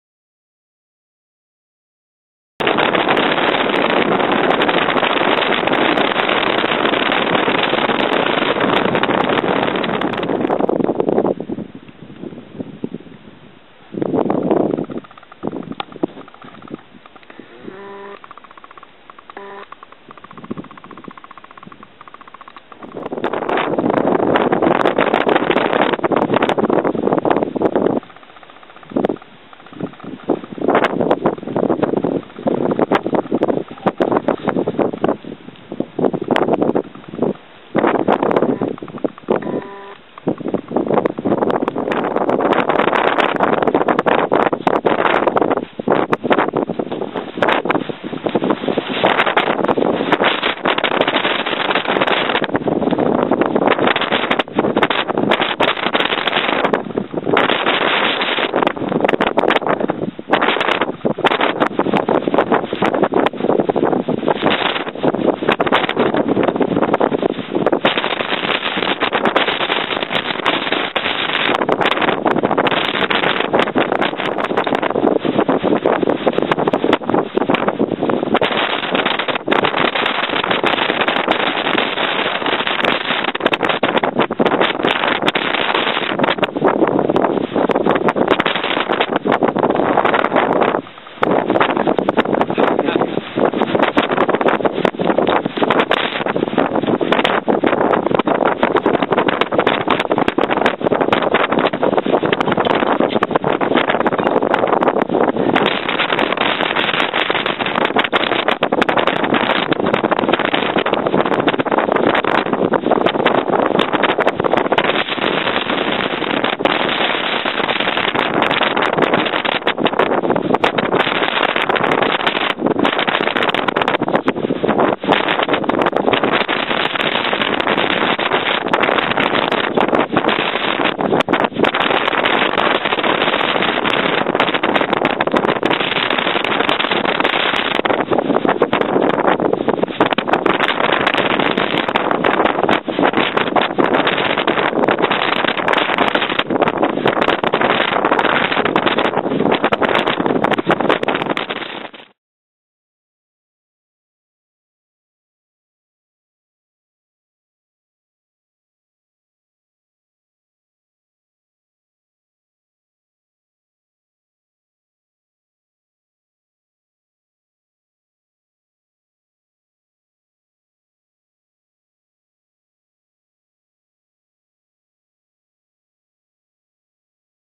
Winter Climbing Report 15th Dec 2008
Climbing Hidden Chimney in windy weather...!